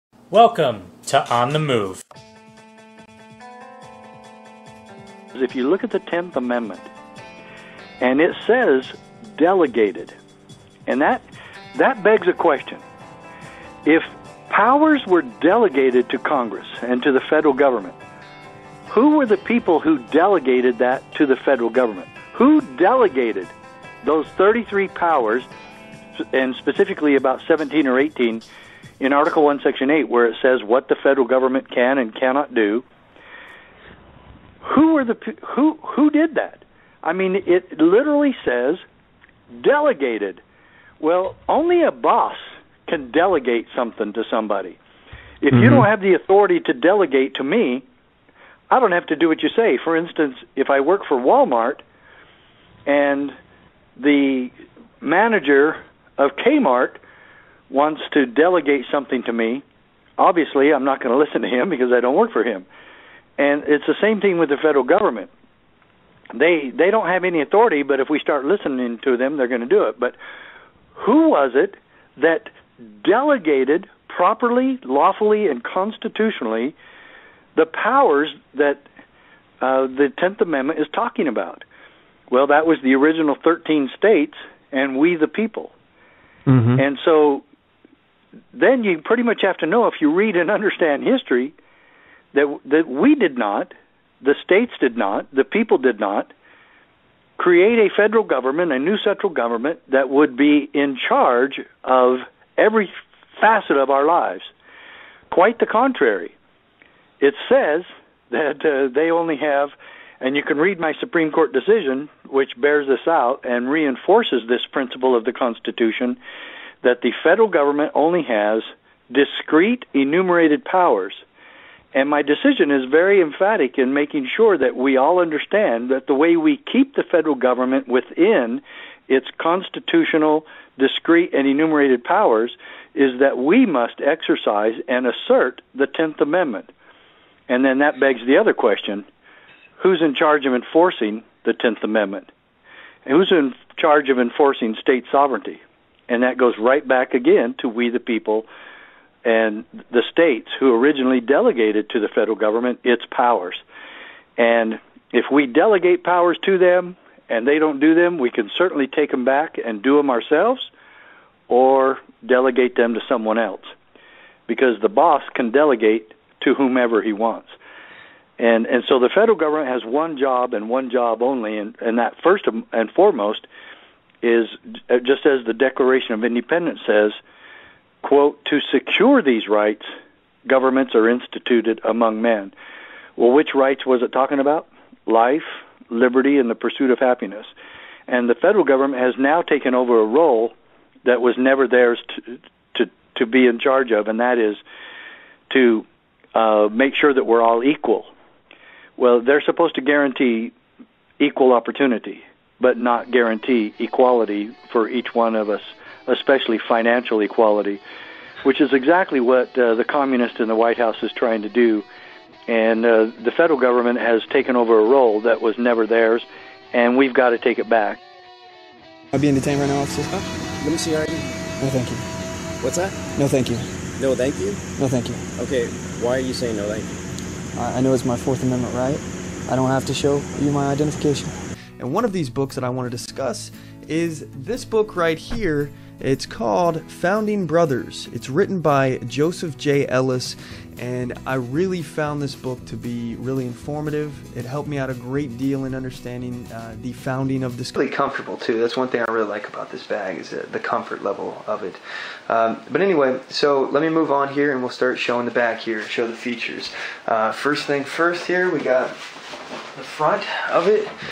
Sheriff Richard Mack | State Rights & Sovereignty | OnTheMoveShow
In this video, Sheriff Richard Mack talks about State rights/sovereignty and the ever expanding federal leviathan. What do you think? Thanks!

Gear, Books, Prepping Gear and MORE: